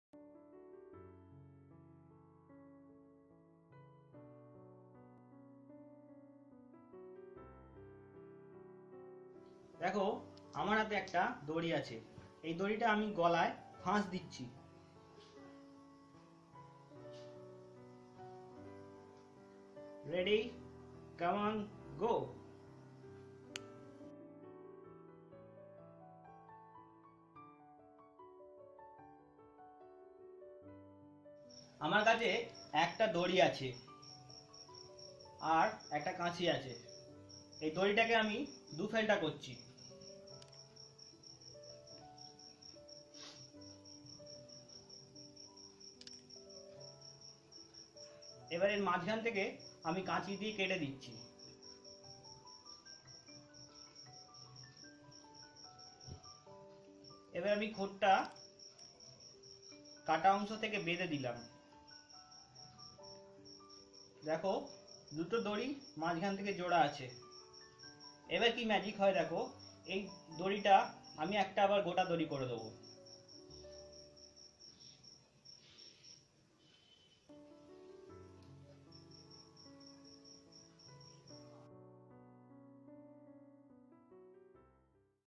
Magic : Rope binding & cutting
2 magic tricks in this video. One rope stacked on neck and release, then a box with rope magic